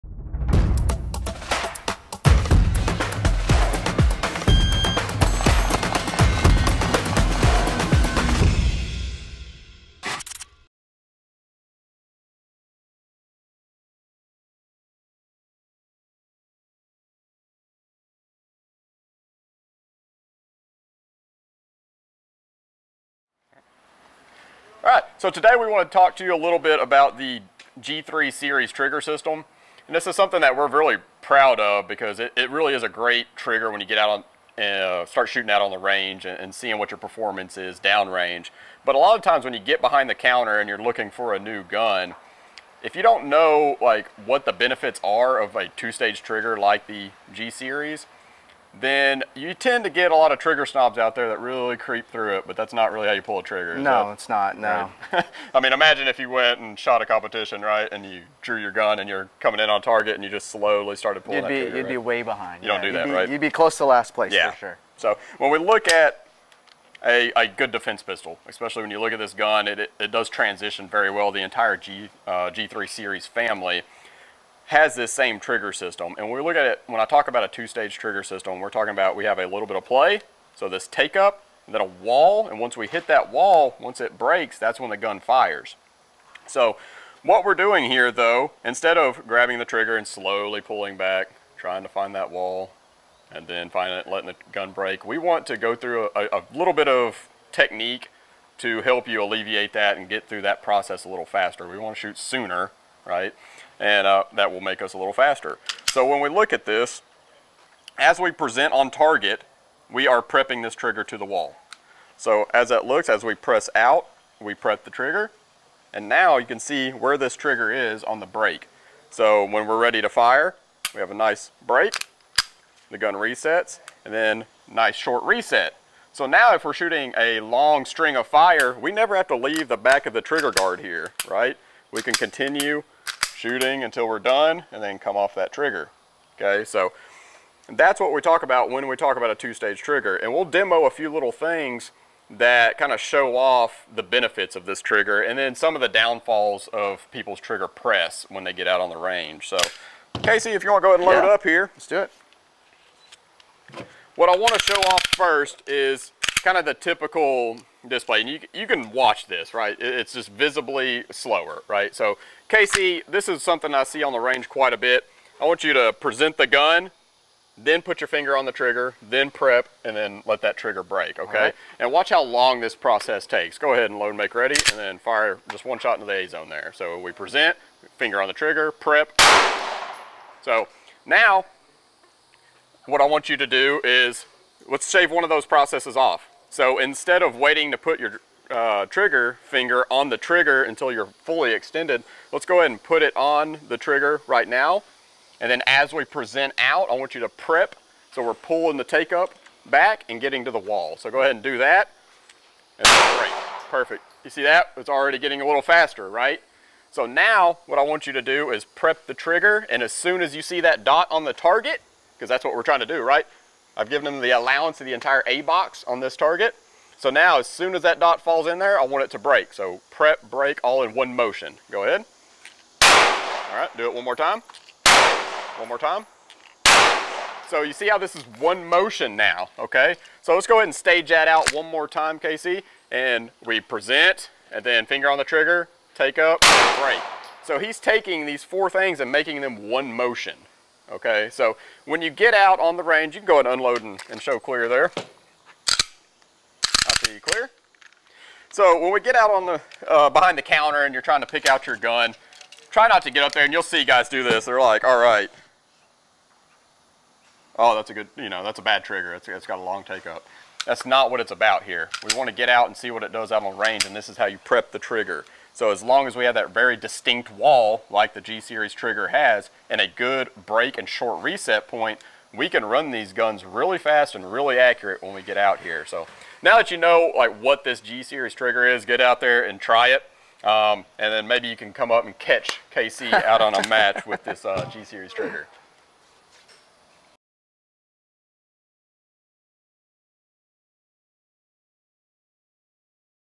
In Depth: Taurus G-Series Trigger System
The hot topic always comes to mind when looking at defensive pistols is the trigger. Let’s face it, we all know when you pick up a new pistol to try out, the first thing we do is rack the slide and test out the trigger. But, if you are new to pistols, some of the striker-fired triggers out on the market may be a little confusing when you first try them out, primarily when most of our first references to a trigger revolve around a hunting rifle.

Hunting rifles typically have a Single Stage trigger, designed to operate in one smooth range of motion. It’s simple, and there is usually little to no take-up involved in using single-stage triggers. The problem with a single-stage trigger in a defensive pistol is that it requires the pistol to have an external safety of some sort to allow the pistol to pass drop testing procedures. Also, this tends to allow for more error in the trigger prep process in high-stress situations.

Trigger prep refers to pulling the trigger up to the point where the resistance builds; this is the point when the trigger is ready to fire the round. This initial section of the trigger pull process is referred to as the “Take-Up” or “Slack” in the trigger.

The G3 series from Taurus uses a Two-Stage trigger system. The Two-Stage trigger is a more common trigger system in defensive pistols on the market. The Two-Stage system allows the gun to utilize a lighter trigger pull and still pass the drop safe procedures, with or without an external safety.

Two-stage triggers have a process that involves two distinct phases of firing. The advantage of this design style is that it allows the user to know precisely when the firing will occur. The first process of the Two-Stage trigger is the “Take-Up”. The Take-Up in Two-Stage triggers usually is quite a bit more when compared to single-stage triggers. Causing trigger snobs who don’t understand defensive trigger systems to complain…This benefit is knowing when the gun will fire, even in high-stress situations. The “Take-Up” is followed by “The Wall” or the Second Stage. Referring to the sudden increase in resistance when the gun is about to release the striker to fire the round. The amount of weight it takes to release the striker is where most pistols on the market fall short, either being too light where the shooter has a strong chance of firing the gun during the prep. Or too heavy, causing the shooting more difficulty when pushing for accuracy.

The G3 trigger system gives you the perfect balance of trigger pull weight for quick and accurate shots while still maintaining a significant amount of safety when needing to hold the trigger at the wall under high-stress situations.

Try out the Taurus G3 family at any local gun store, and remember to get training. You can never learn enough, and spending time on the range with like-minded people working to get better is always fun.